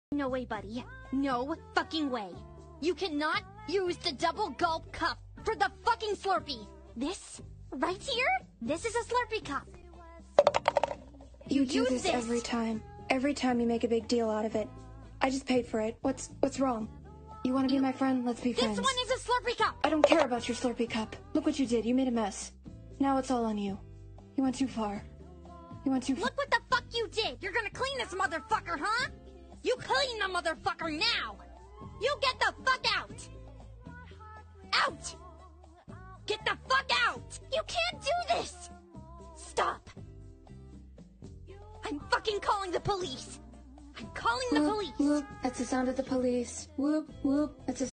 The Elevator Scene but they're fighting about the double gulp cup
Credit to whoever made this